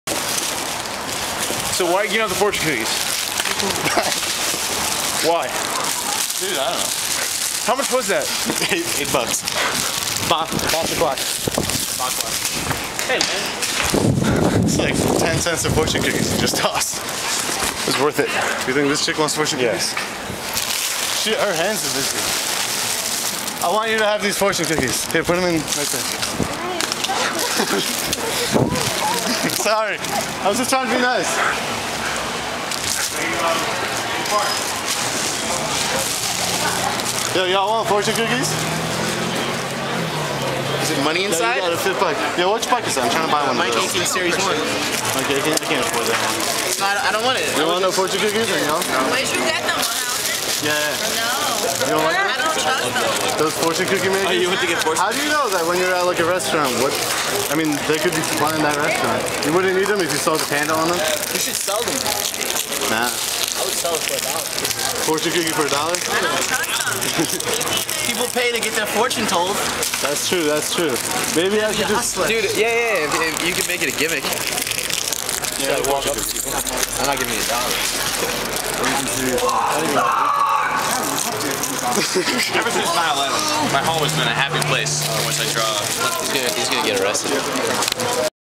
Fortune Cookies
Borna bought a box of fortune cookies to share with his fellow New Yorkers